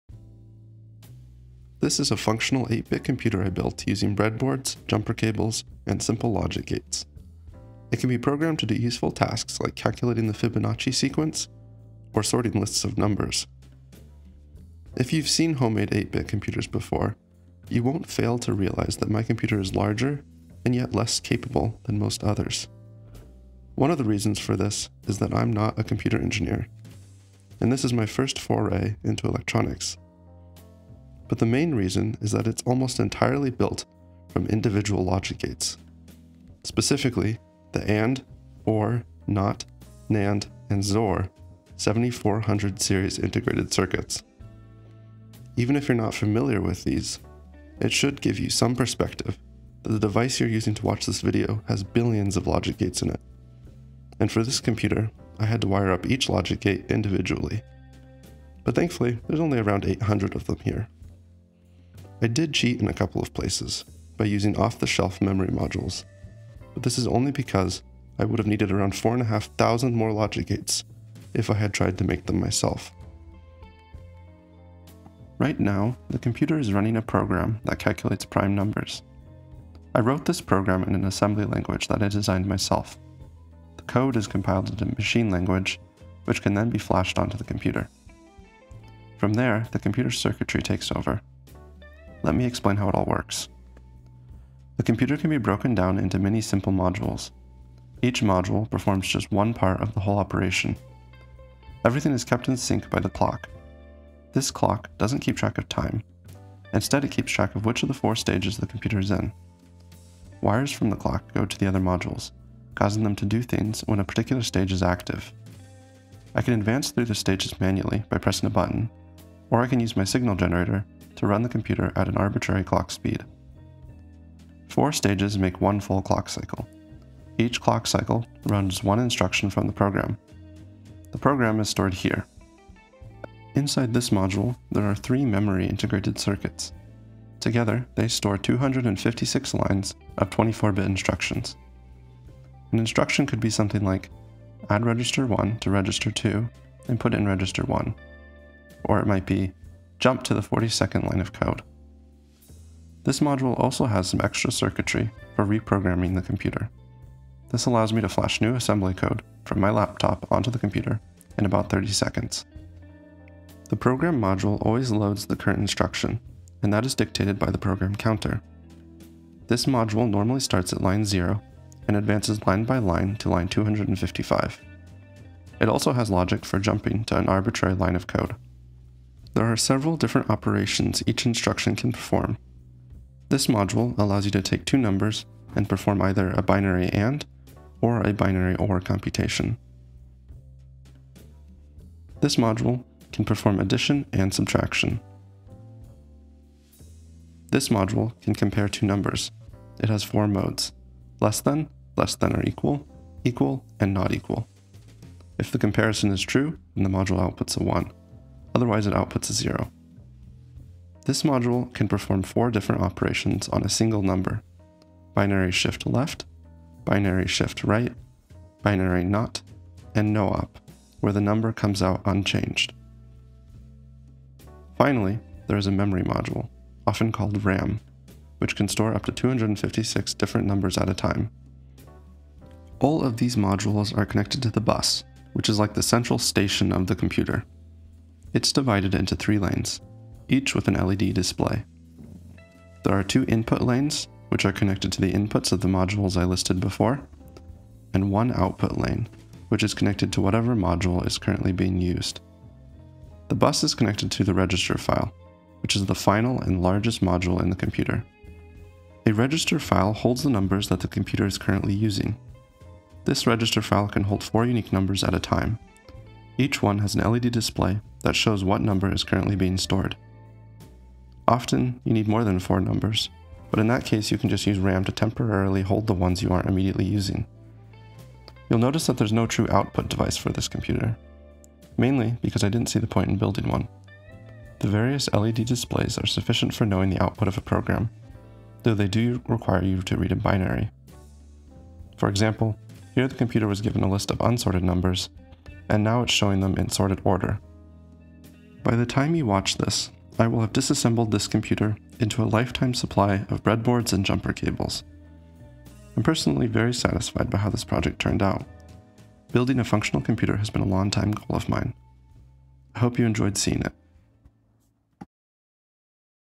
Homemade 8-bit Computer
Showing off my homemade 8-bit computer. I built this using only basic 7400 series integrated circuits. It can't do much, but building a computer from such low-level components was a bucket-list item for me. Hope you enjoy seeing it as much as I enjoyed building it!

Some factoids:

Total material cost: ~$400
Max clock rate: ~0.5 MHZ
Total number of breadboards: 36
Number of jumper cables: I lost count, but Amazon was delivering a couple packs a week.
Most difficult module to build: The program memory (the chips I used have a very complicated procedure for writing to them).
Easiest module to build: The memory and the register file.
Number of months I haven't been able to use my kitchen island: 3